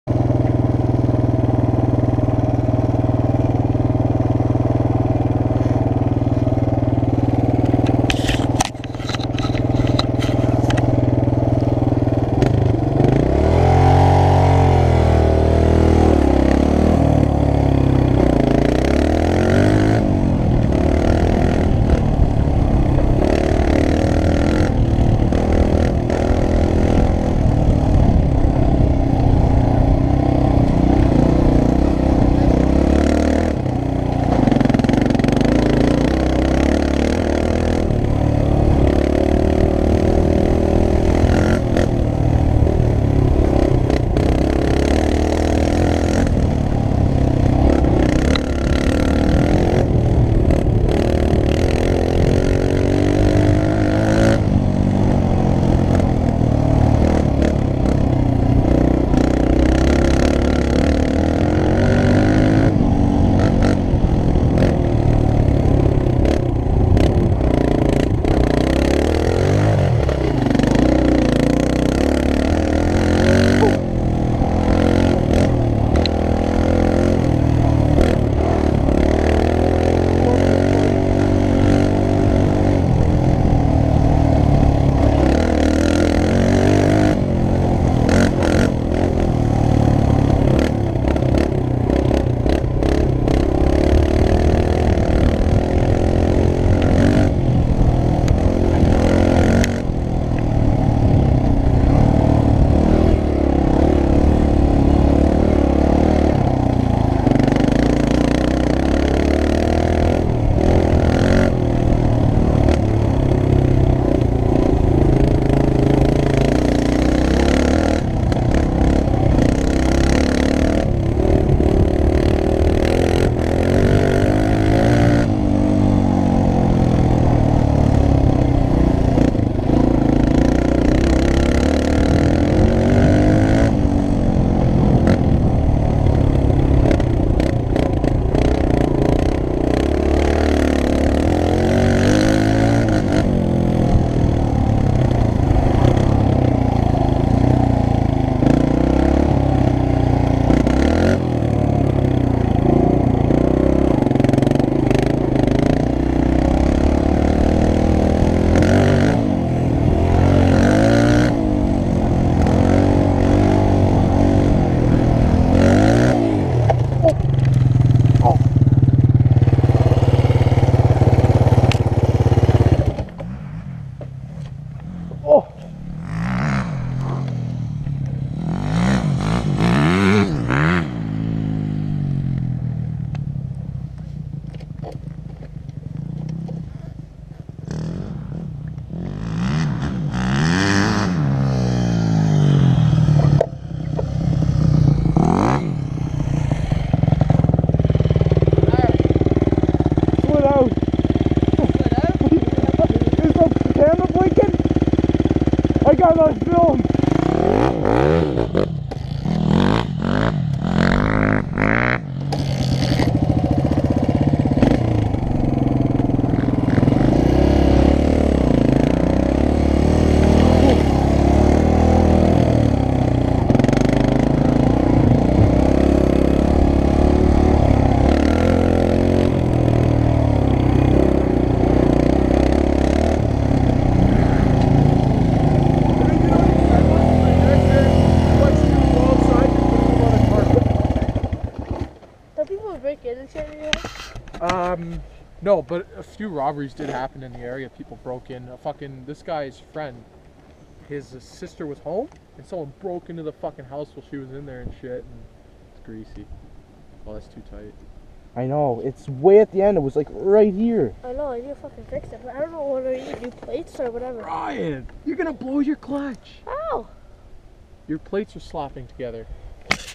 Drz125 Pit bike ride at homemade track , Crash
2013 drz125 pit bike at homemade track and I crashed

this is my friends bike